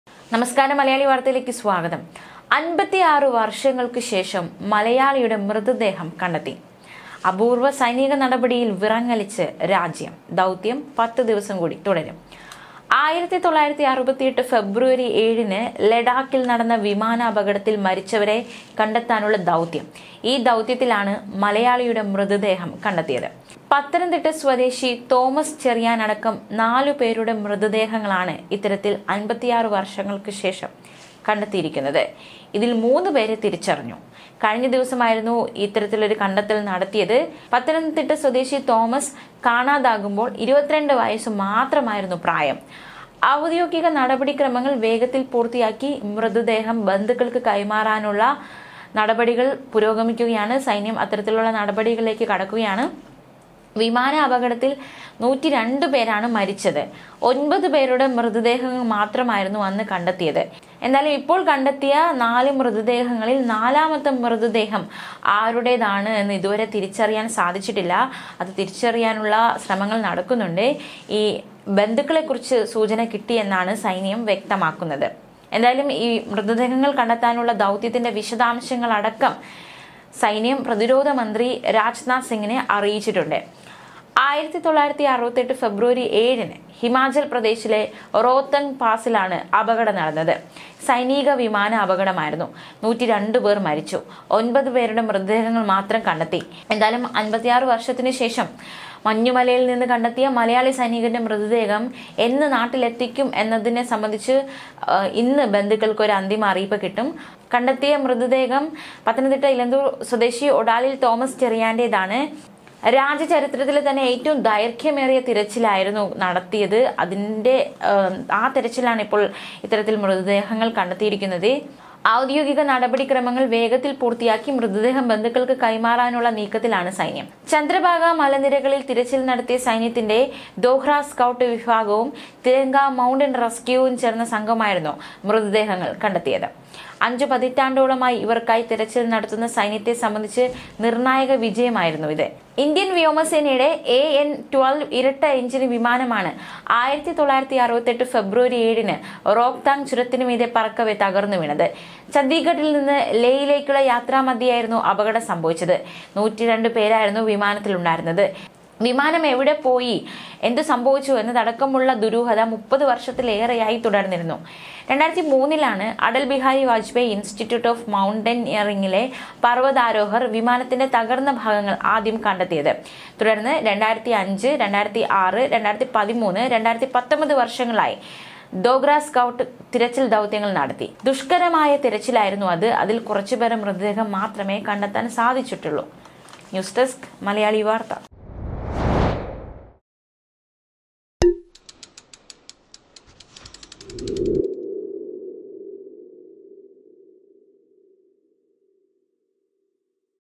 56 വർഷത്തിന് ശേഷം | INDIAN ARMY | INDIA
​ @malayalivarthakerala 2024
"56 വർഷത്തിന് ശേഷം
മഞ്ഞുമലയിൽ മൃത ദേഹം...!
സൈന്യം കണ്ട കാഴ്ച്ച!
വിമാനാപകടത്തിൽ തകർന്നത് "